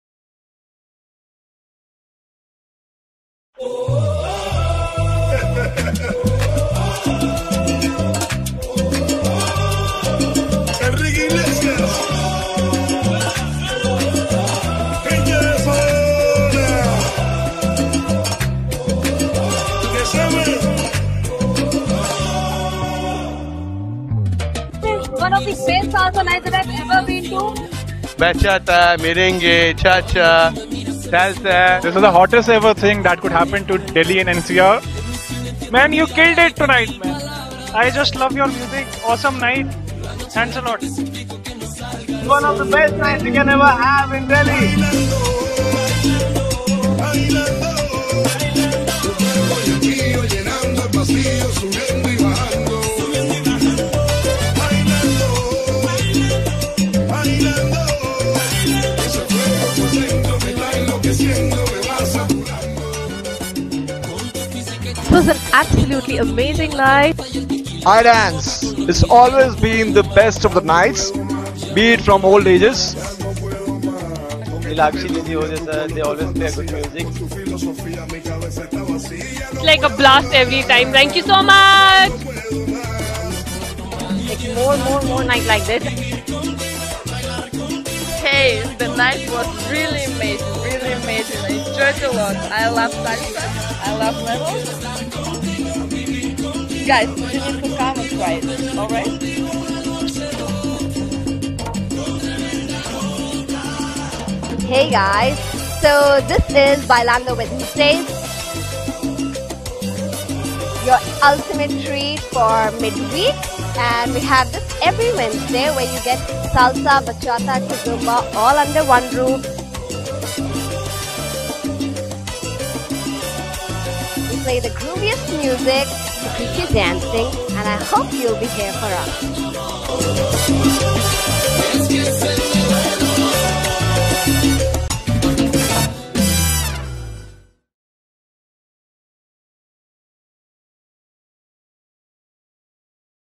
Salsa Nights of World
BAILANDO WEDNESDAYS
Organized by iDance India, Neelakshi, Jose Varghese
Dance Night playing Salsa, Bachata, Kizomba
Happening Every Wednesday, 9.30 PM - 12.30 AM
Popular night of New Delhi
Happening At LEVELS, Hauz Khas Village
Open for Everyone familiar with salsa, bachata, kizomba.

*Club Rules Apply
* Entry Strictly by guest list.
To Attend Send Names to - # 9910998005